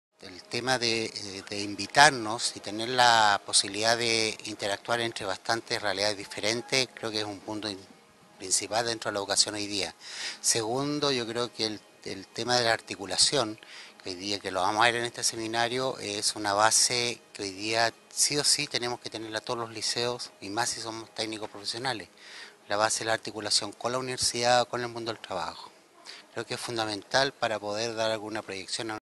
José Salum, Dir. Complejo Educ. Monseñor Guillermo Hart
José Salum, Director Director Complejo Educacional Monseñor Guillermo Hartl, en el XVI Seminario Claves para la Educación del Futuro, realizado por Fundación Irarrázaval el 31 de julio y 01 de agosto de 2015.